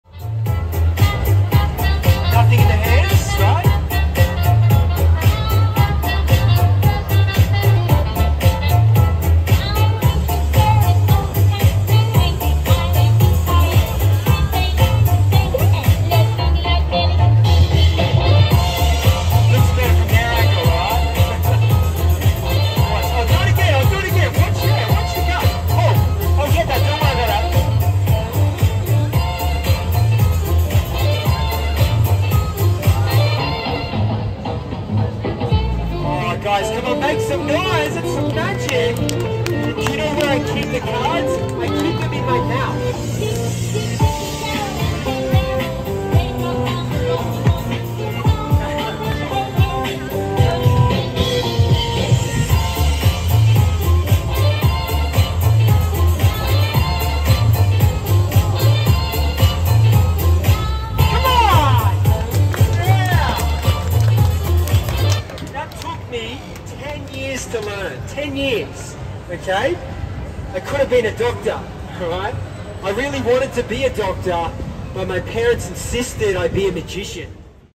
STREET MAGIC IN CAVILL MALL SURFERS PARADISE//streetentertainment
In this video, you can watch this awesome street magician performing a magic trick with his cards :)